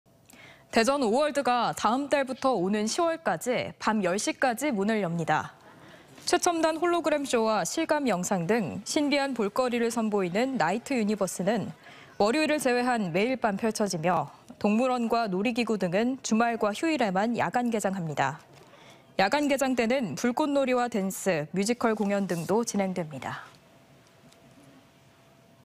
대전 오월드 다음 달부터 10월까지 야간 개장｜ TJB 대전·세종·충남뉴스
대전 오월드가
다음 달부터 오는 10월까지
밤 10시까지 문을 엽니다.

최첨단 홀로그램 쇼와 실감 영상 등
신비한 볼거리를 선보이는 나이트 유니버스는
월요일을 제외한 매일 밤 펼쳐지며,
동물원과 놀이기구 등은
주말과 휴일에만 야간 개장합니다.

야간 개장 때는 불꽃놀이와 댄스, 뮤지컬
공연 등도 진행됩니다.